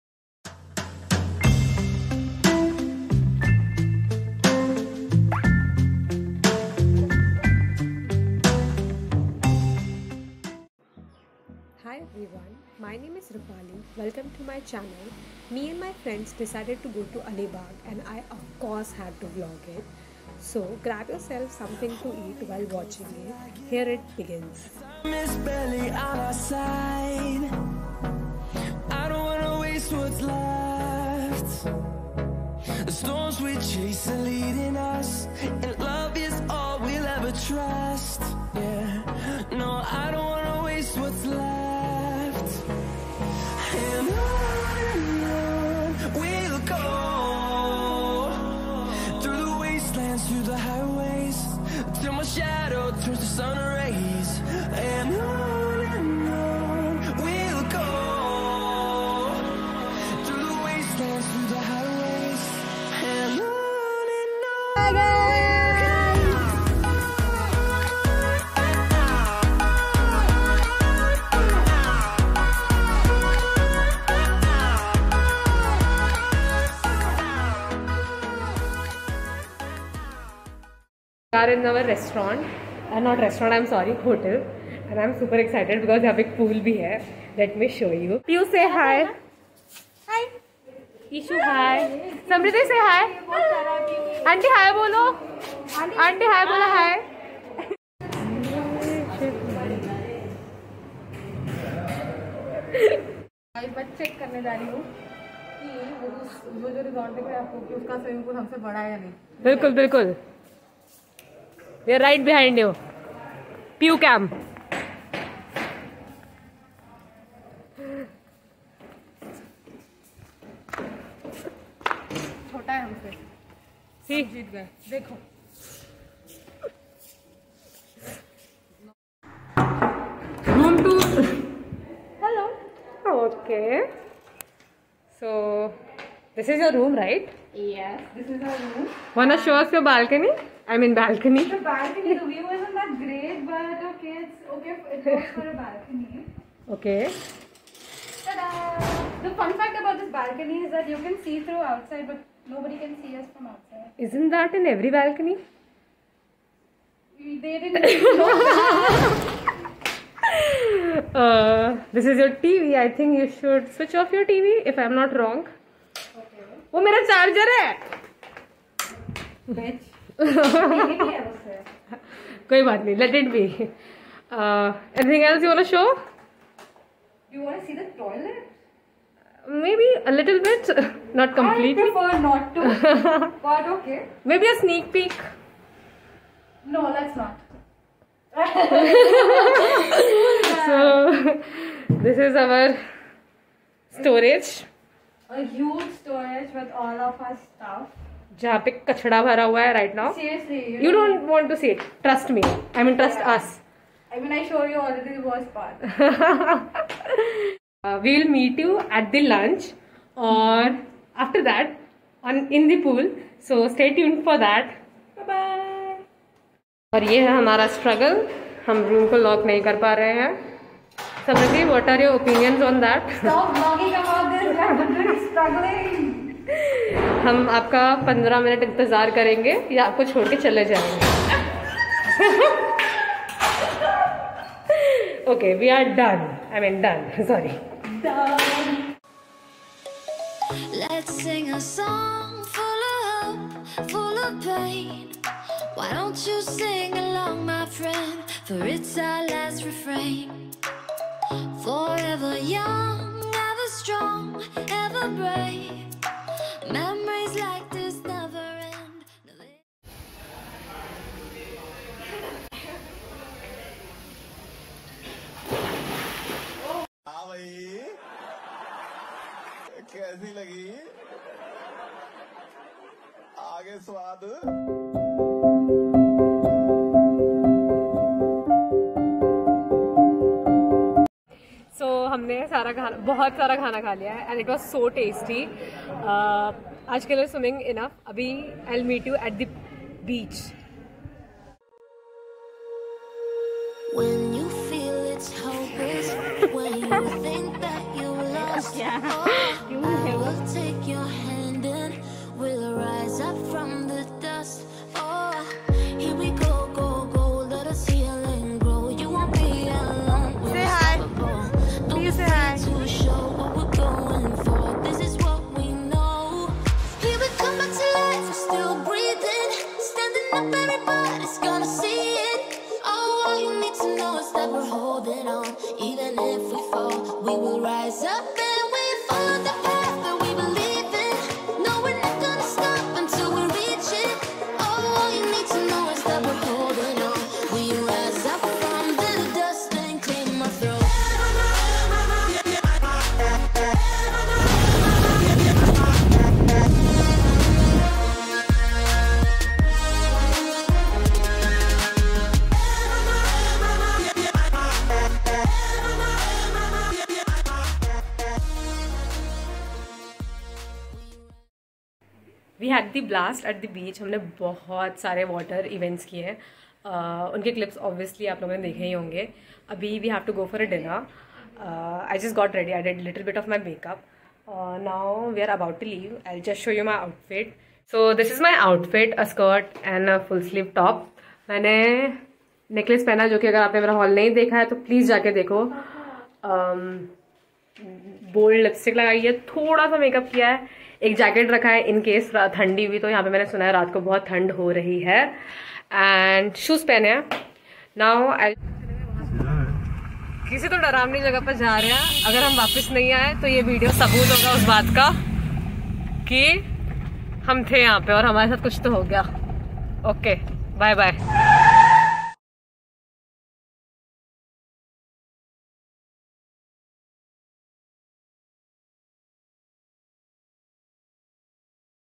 Alibaug Vlog Part 1| My first vlog| with Friends|swimming|Water activities | Nagoan Beach
I love watching vlogs  and making them but because of corona it was almost impossible .Finally when me and my friends decided to go to alibhaug I decided to vlog the entire trip.
It's my first vlog please let me know what you feel about it. The second part will be up within few days.
Also this is my first time editing. Till now my friend Hitesh was editing my videos. Let me know if you liked or disliked the editing.
Music credits-

Copyright:-
Everything you see on this video was created by me (Rupali ) unless otherwise stated.